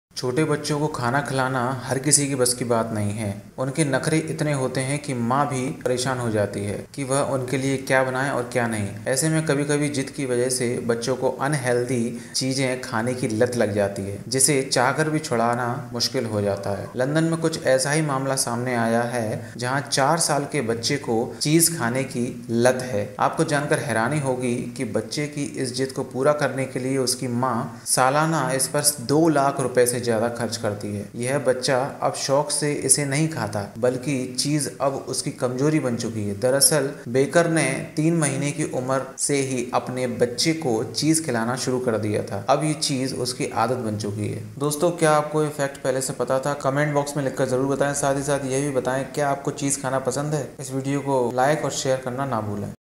बच्चों की Unhealthy खाने की लत
Hello Friend's

Feeding small children is not a matter of everyone's tantrums, their tantrums are so much that even the mother gets upset about what to make for them and what not, in such a situation, sometimes due to stubbornness, children are addicted to eating unhealthy things. It becomes difficult to get rid of it even if you want Spends more than ₹ 200000 on this, this child no longer eats it with passion, but the thing has now become his weakness, in fact Baker started feeding cheese to his child from the age of 3 months, now this thing has become his habit. Is

छोटे बच्चों को खाना खिलाना हर किसी के बस की बात नहीं है उनके नखरे इतने होते हैं कि मां भी परेशान हो जाती है कि वह उनके लिए क्या बनाएं और क्या नहीं ऐसे में कभी कभी जिद की वजह से बच्चों को अन हेल्थी चीजें खाने की लत लग जाती है जिसे चाह कर भी छुड़ा पाना मुश्किल हो जाता है लंदन मैं कुछ ऐसा ही मामला सामने आया है जहां 4 साल के बच्चे को चीज खाने की लत है आपको जानकर हैरानी होगी कि बच्चे की इस जिद को पूरा करने के लिए उसकी मां सालाना इस पर ₹200000 से ज्यादा खर्च करती है यह बच्चा अब शौक से इसे नहीं खाता बल्कि चीज अब उसकी कमजोरी बन चुकी है दरअसल बेकर ने 3 महीने की उम्र से ही अपने बच्चे को चीज खिलाना शुरू कर दिया था अब यह चीज उसकी आदत बन चुकी है